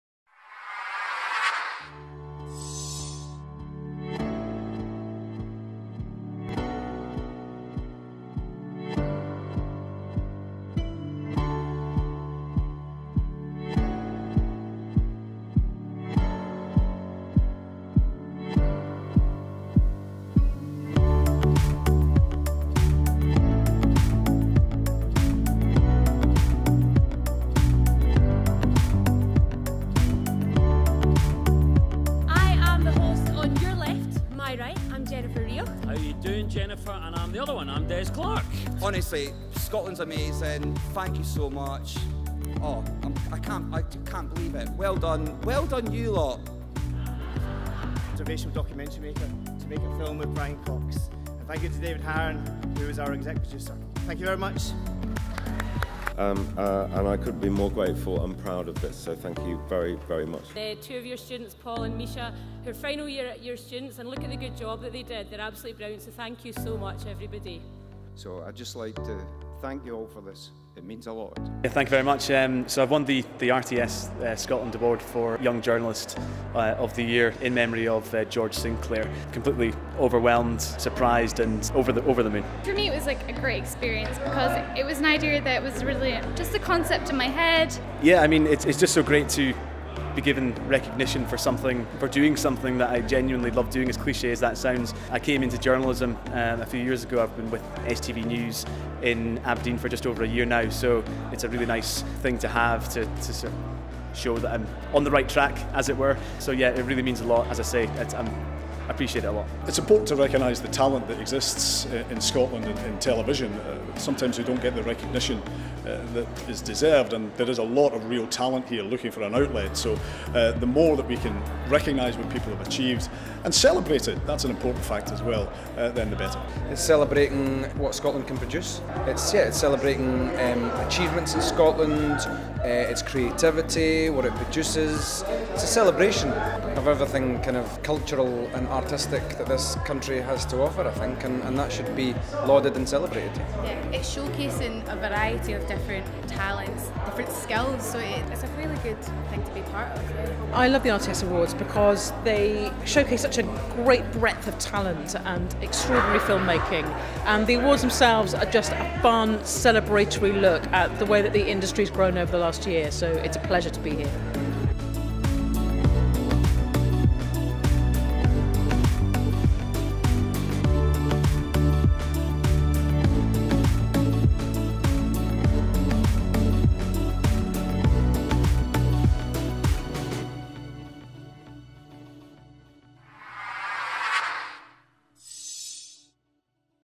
RTS Scotland Awards 2018 | Highlights
Watch highlights from this year's RTS Scotland awards, celebrating the nation's best television from the past year.